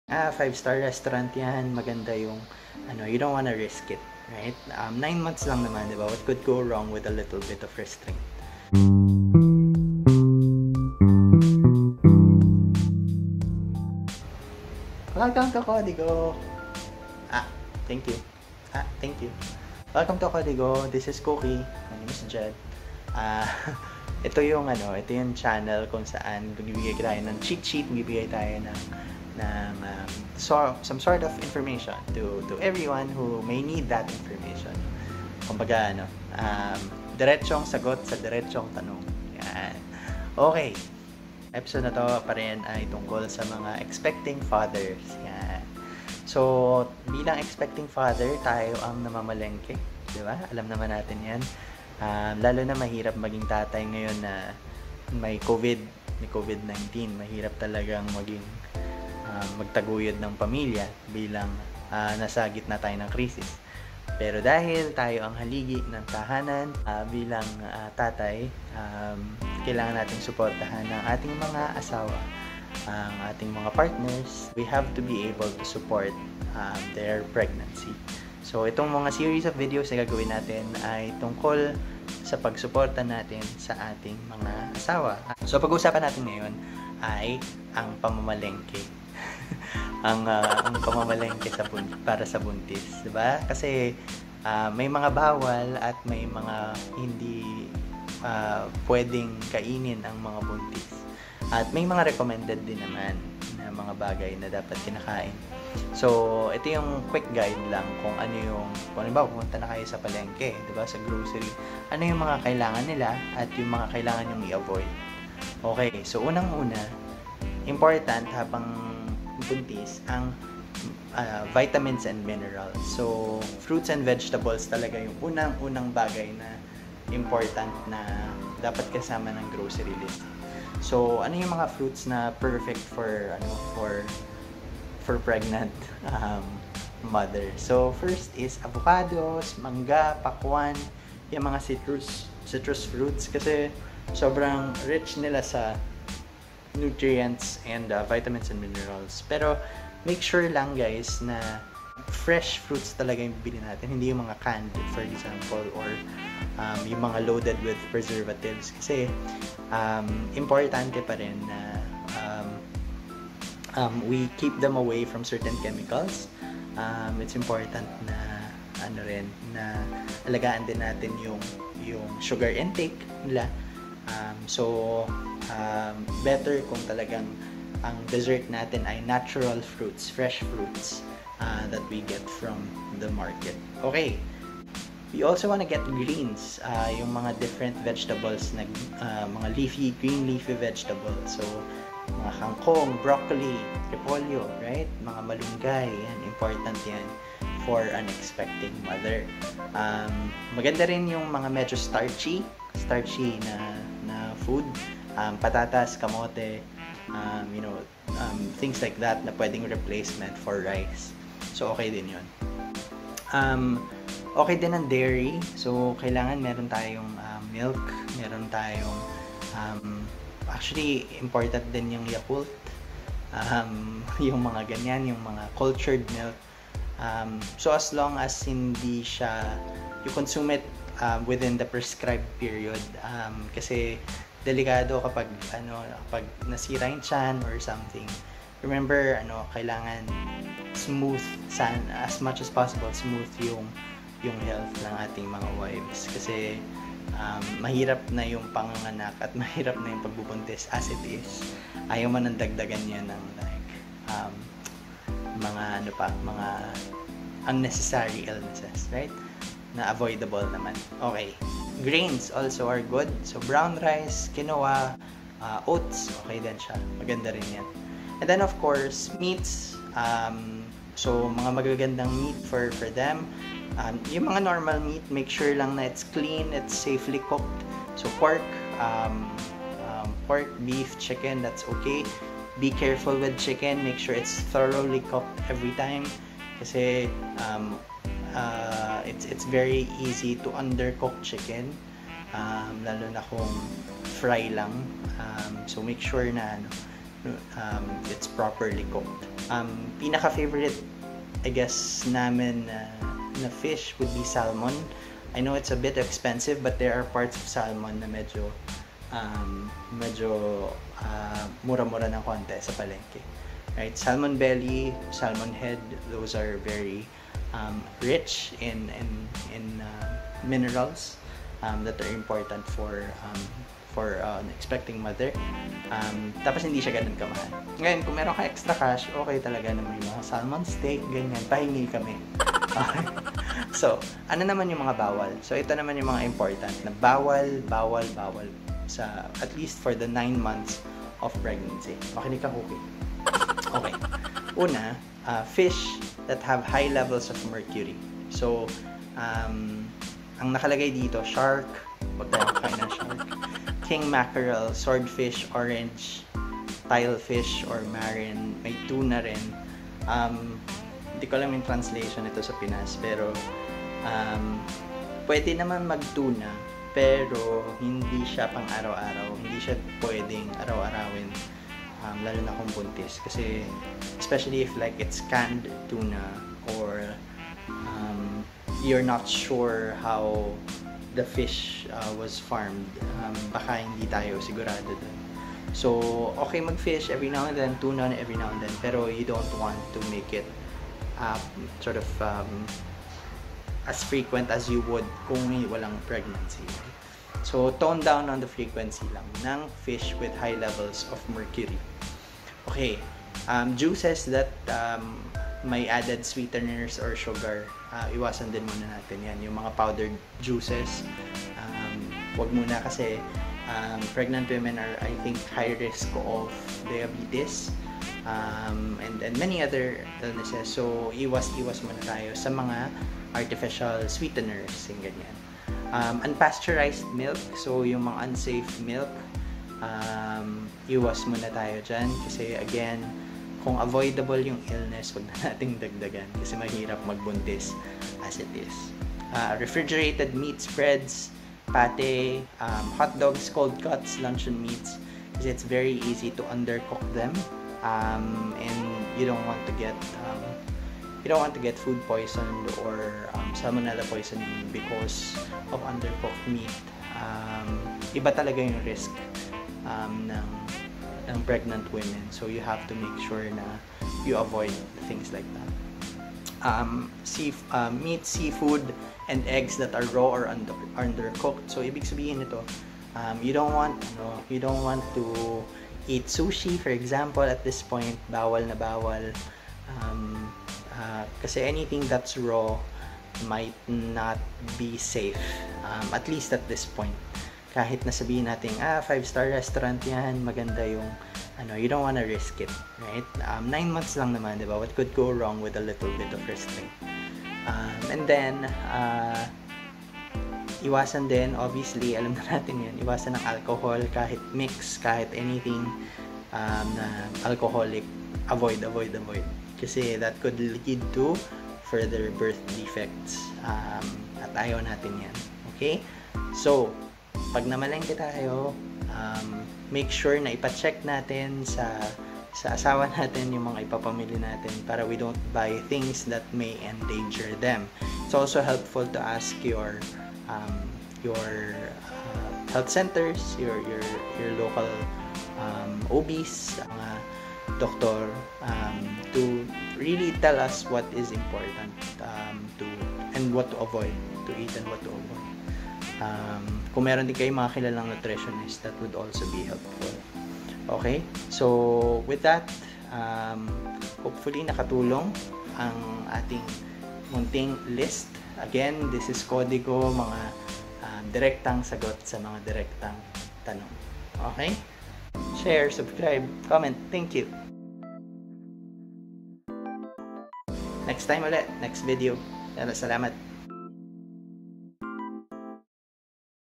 Groceries Para Sa Mga Buntis- Palengke List for Pregnant Women (Tatay Tips)
Ano ang pwede at hindi pwedeng kainin ng mga buntis?